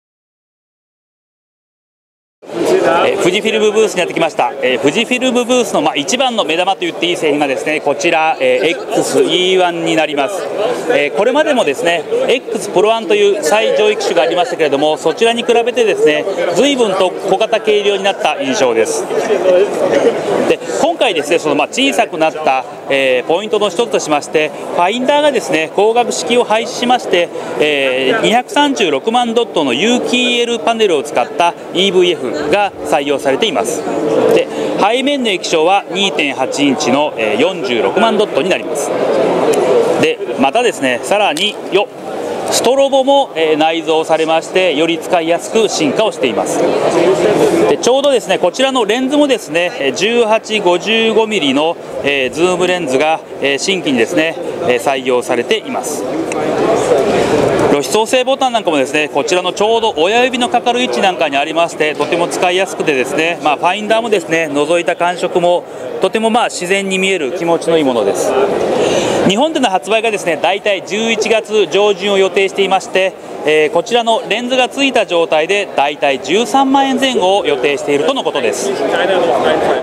【フォトキナ2012】富士フイルム X-E1
富士フイルムは、ミラーレスデジタル一眼カメラの新製品「X-E1」を発表した。詳しくはこちら